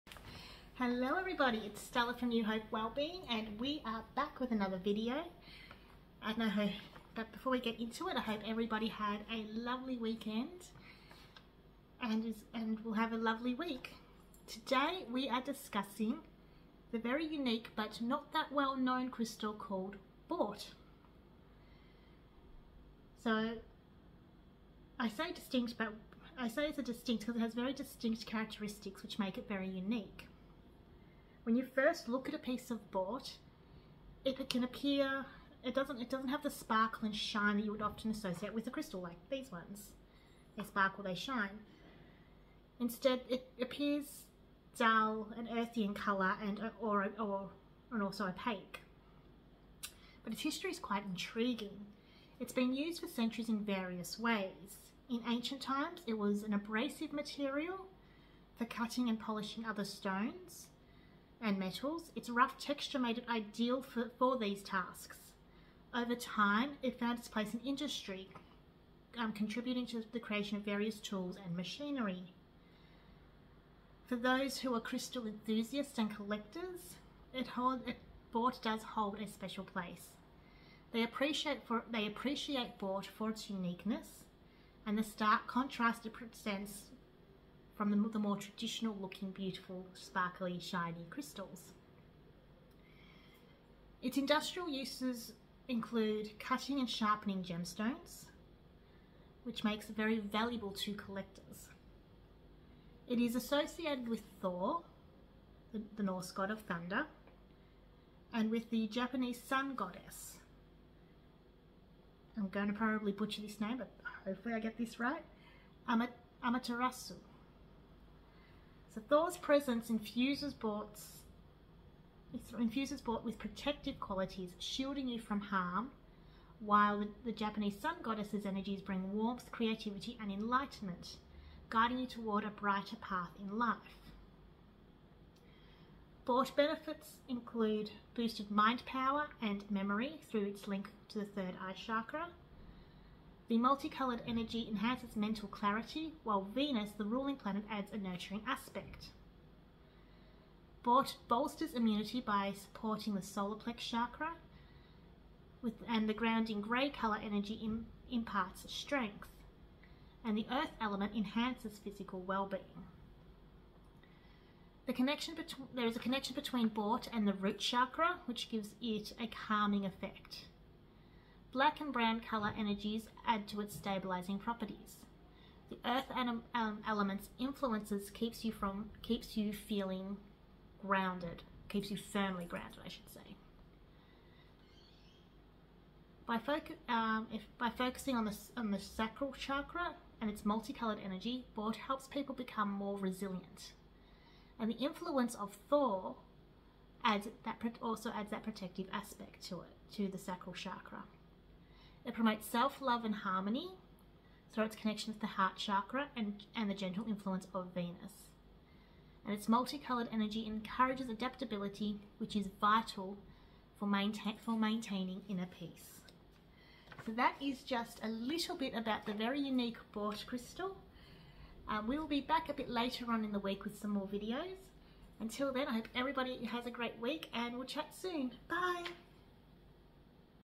Bort Crystal
Today we discuss the benefits of the unique Bort Crystal